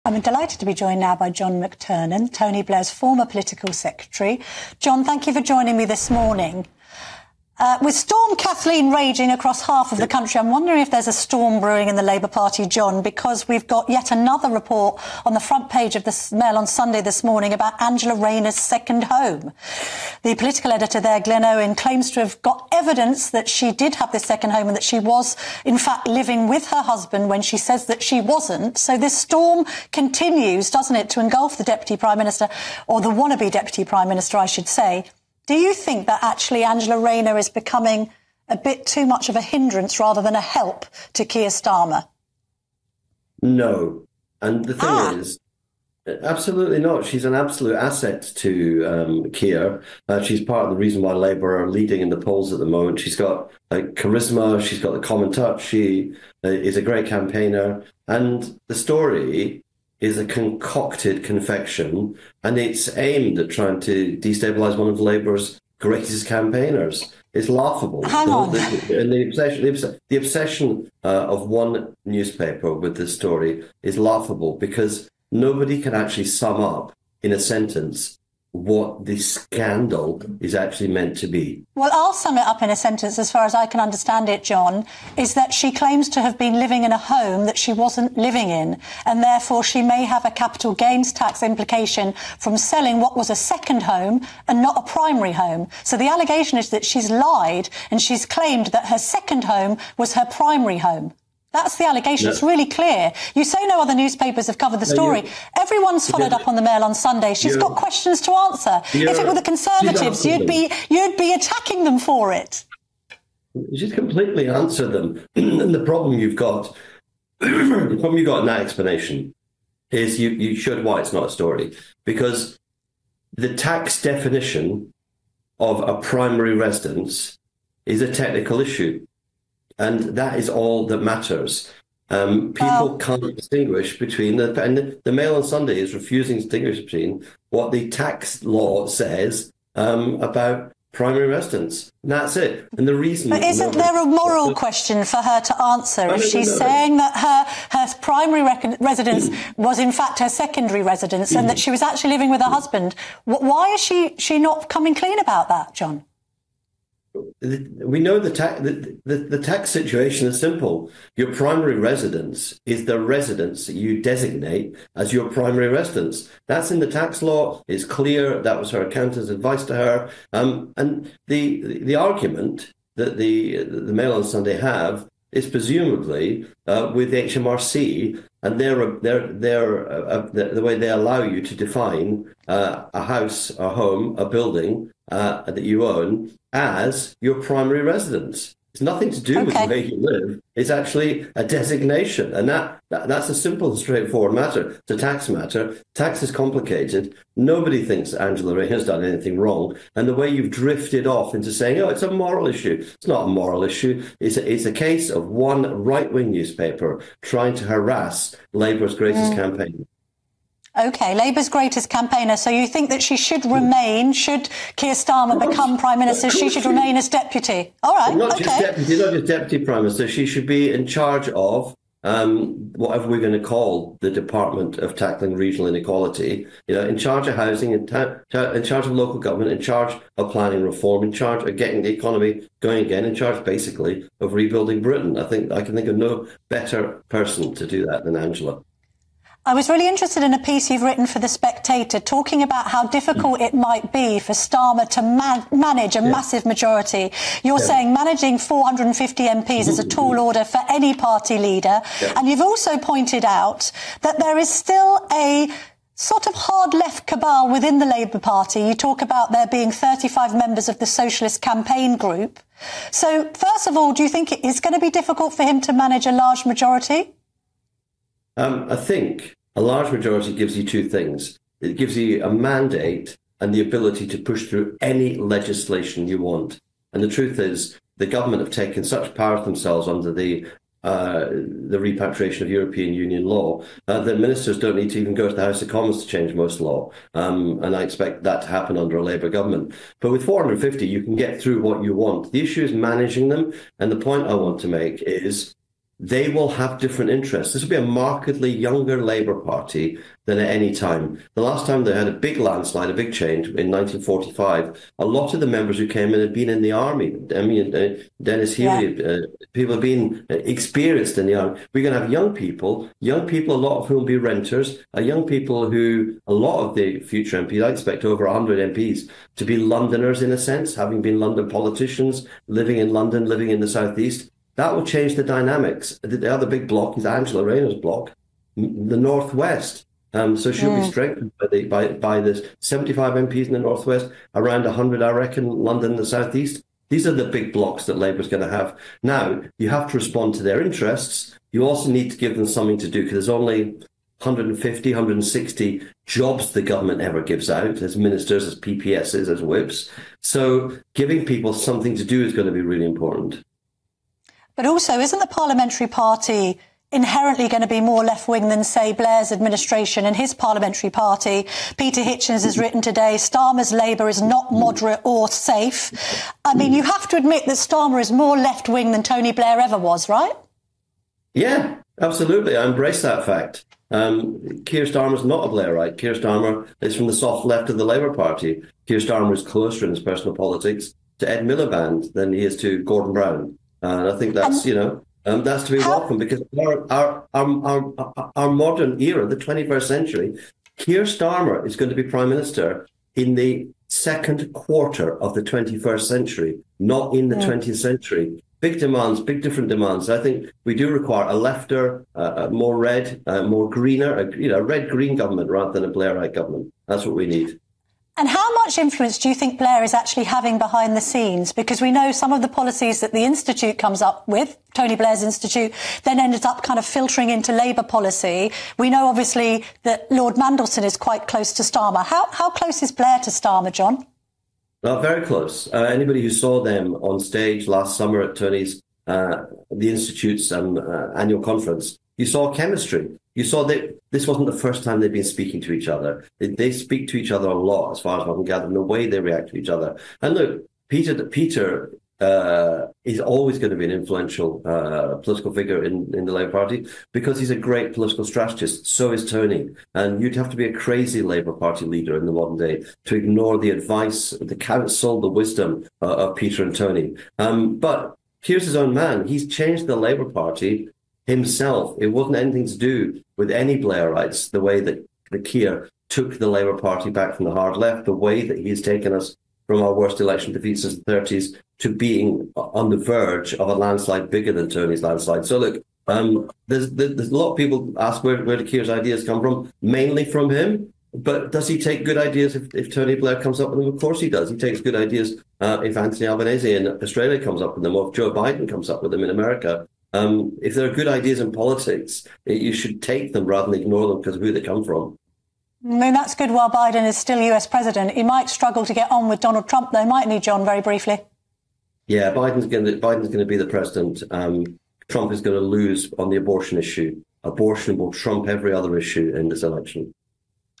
Angela Rayner 'should be in charge of rebuilding Britain' - 'A real asset to Keir Starmer'
Absolutely not! She is an absolute asset to Keir….she should be in charge of rebuilding Britain!’

Camilla Tominey asks Former Labour Adviser, John McTernan, if he thinks Angela Rayner is a ‘hinderance’ to Keir Starmer following her tax scandal.